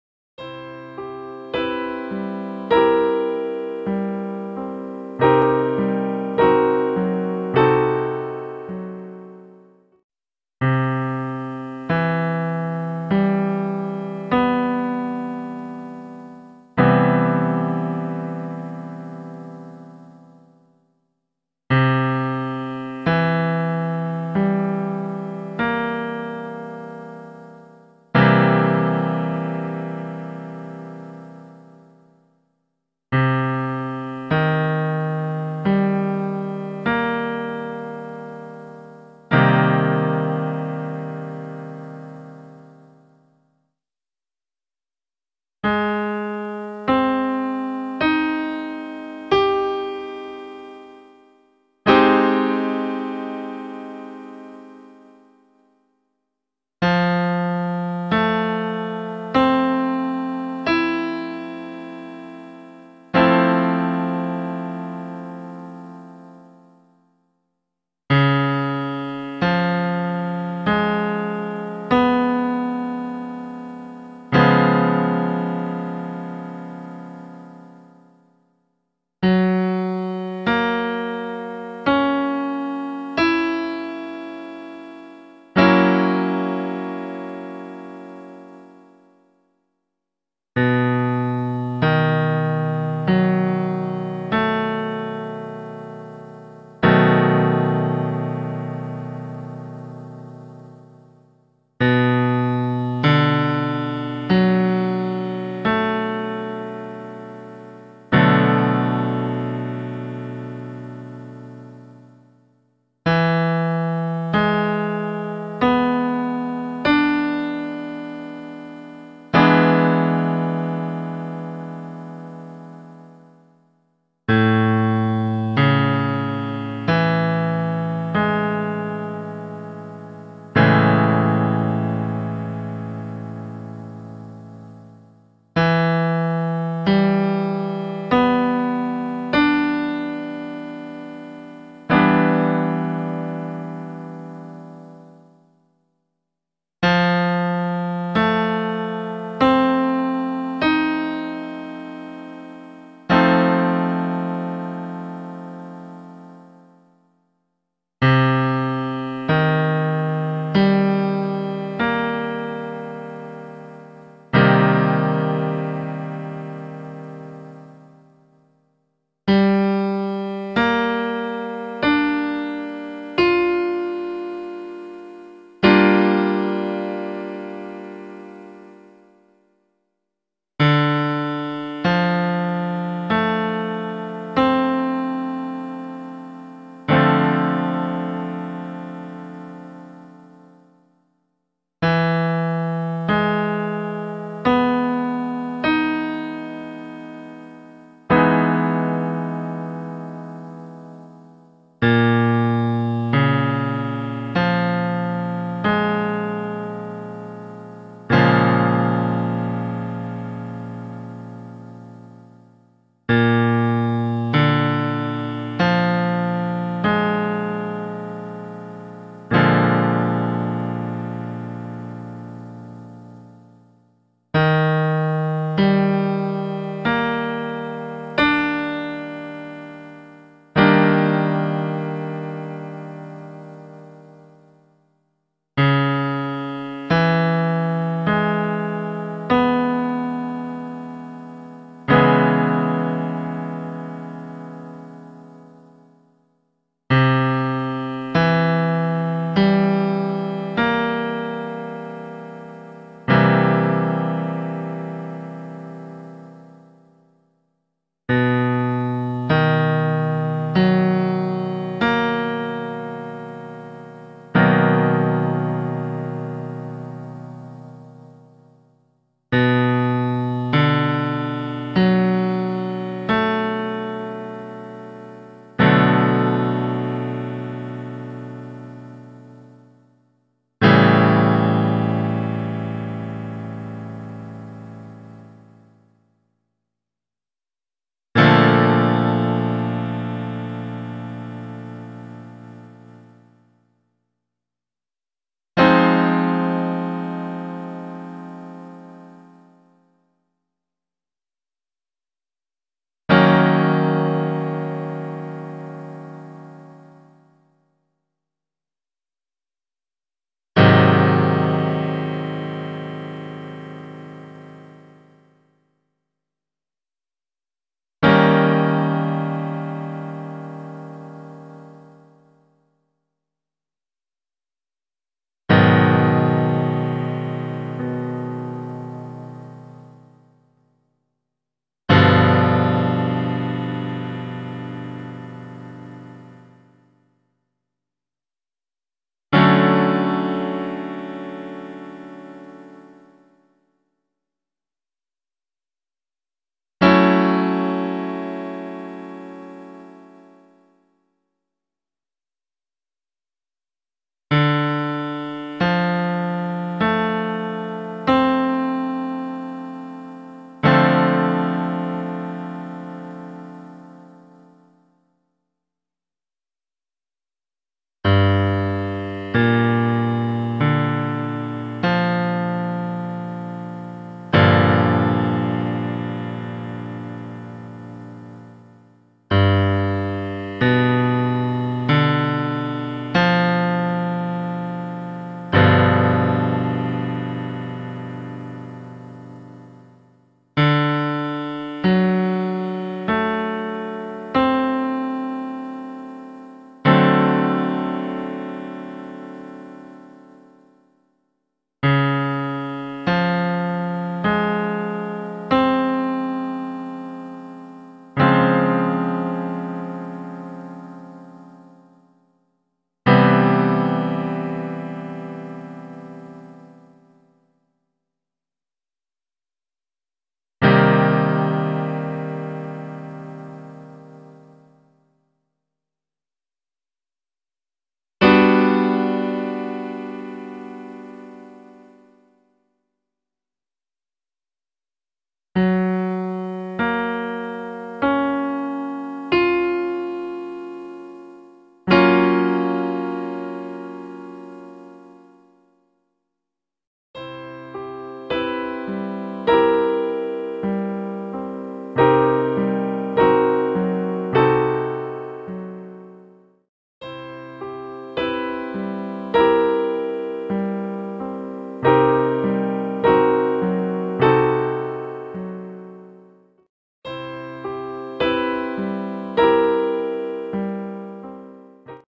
02 - Percepção TÉTRADES - 7M, 7 & m7
Gabarito no fim do vídeo.
(Aplicando a técnica do "dó móvel"(Maior) & "lá móvel"(menor)